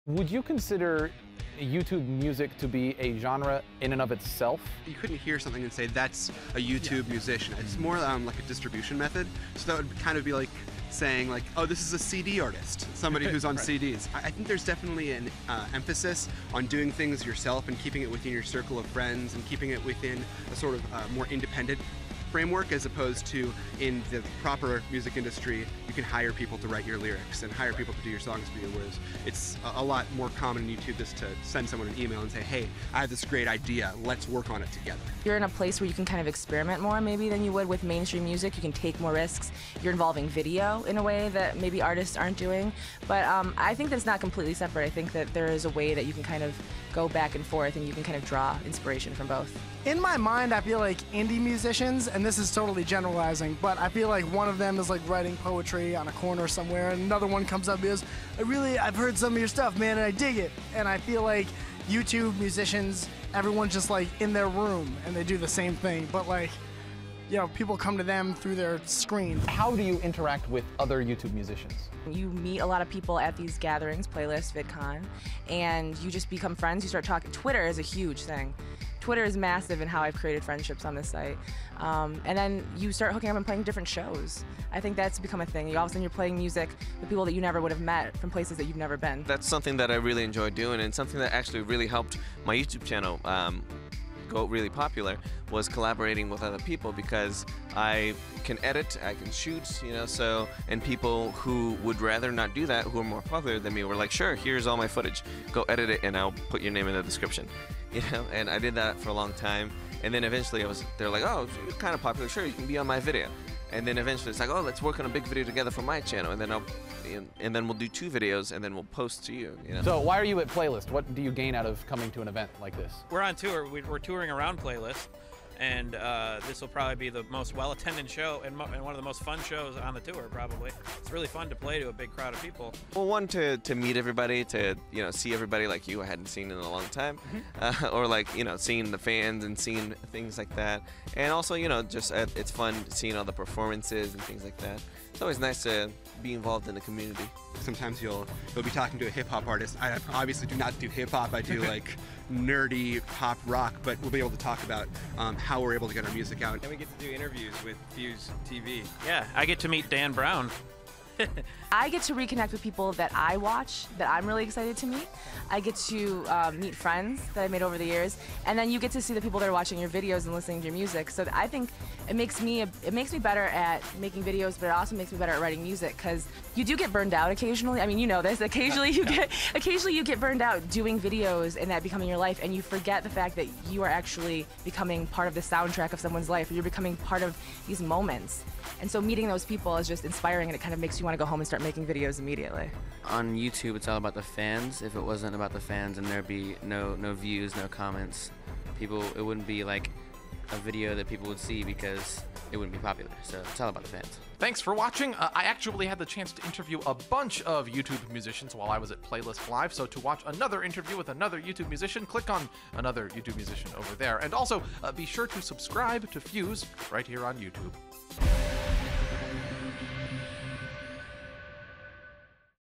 Advice From YouTube Musicians - Playlist Live 2012
We asked artists from Playlist Live 2012 in Orlando, FL to talk about what it takes to be a YouTube musician.